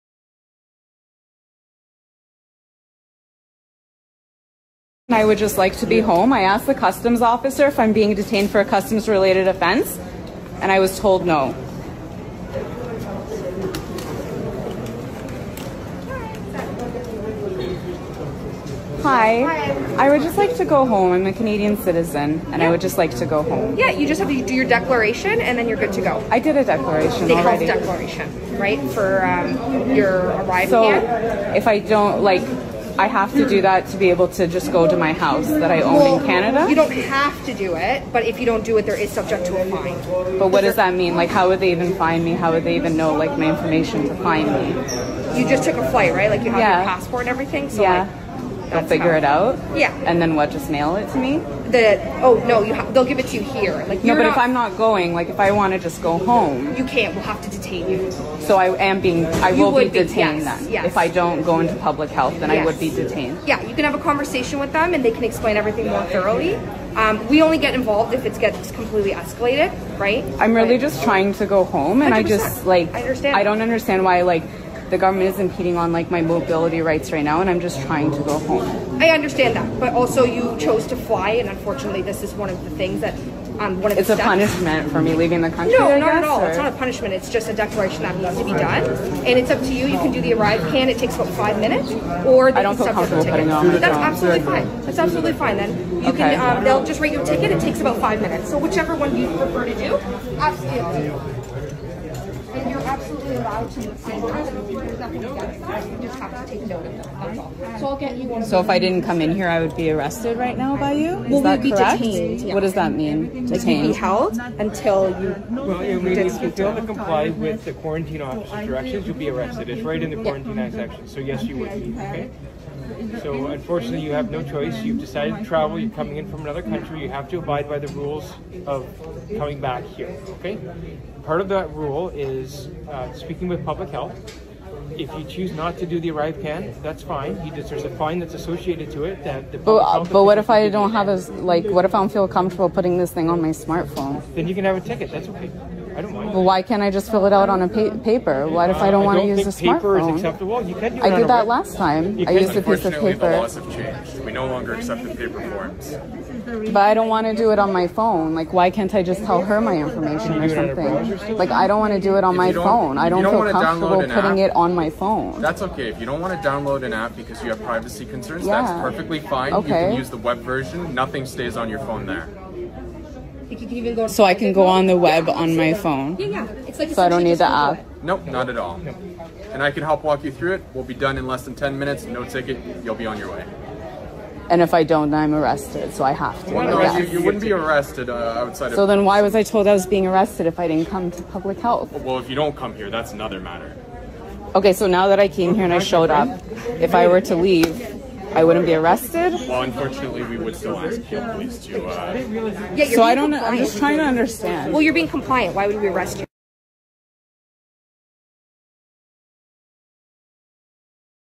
Police Help Public Health Detain & Fine Canadian Traveler Refusing to Use ArriveCan App | Sep 8 2022
Police Help Public Health Agents Detain & Fine a Canadian Traveler for Refusing to Use the ArriveCan App. The officers stop the traveler from returning home as a Canadian citizen. She simply wants to go home, and refuses to talk with the public health agents after completing her regular immigration checks. Protests and police simply doing their jobs continue across Canada.

BrightCanNews is a Project of BrightWorks